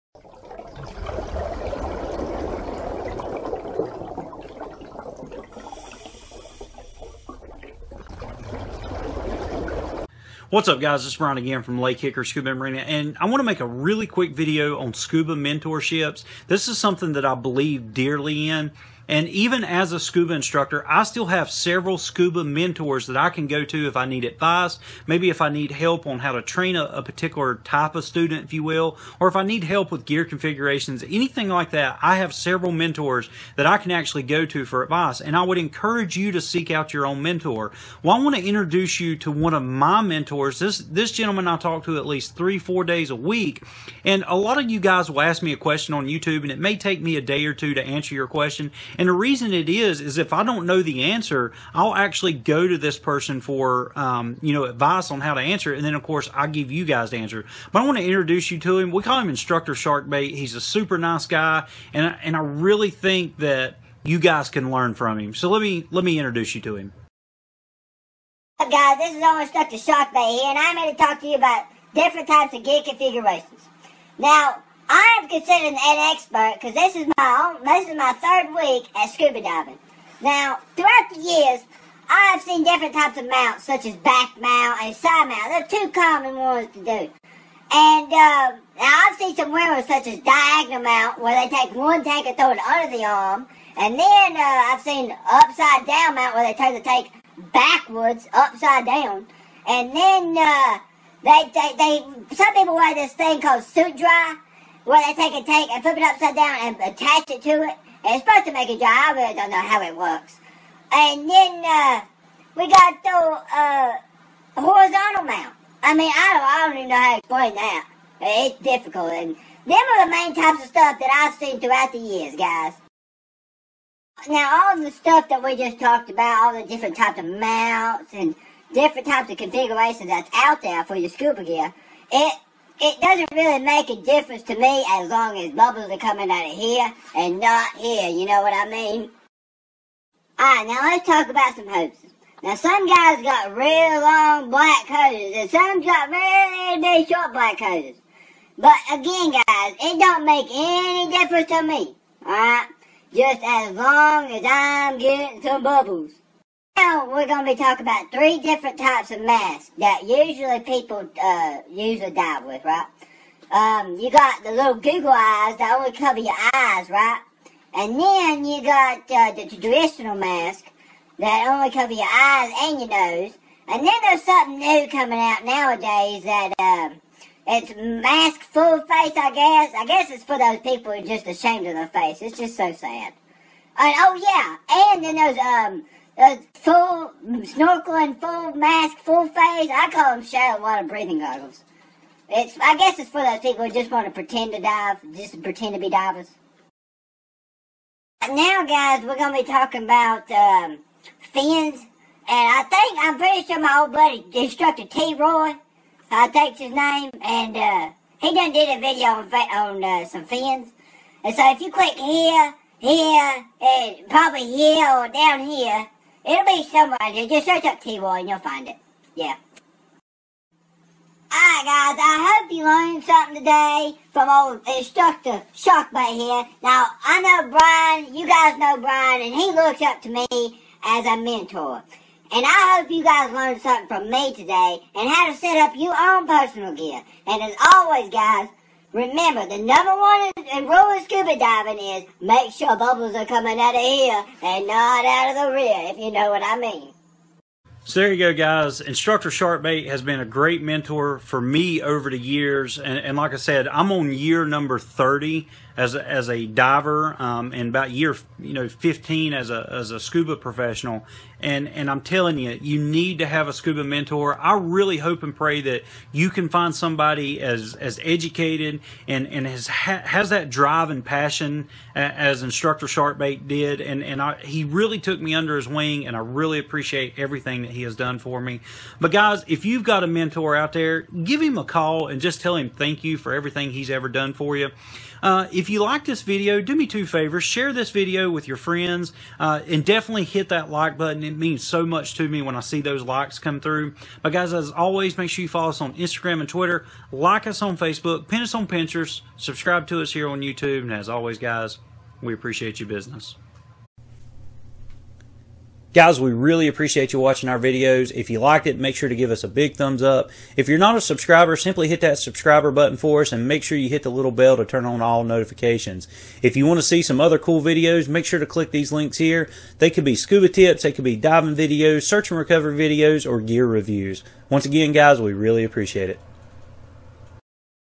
Even Dive Professionals Have Mentors
I have been very blessed to have many great Scuba Mentors over the years, but one has really stood out above all the rest.  Instructor Shark Bait is the Mentor of all Mentors.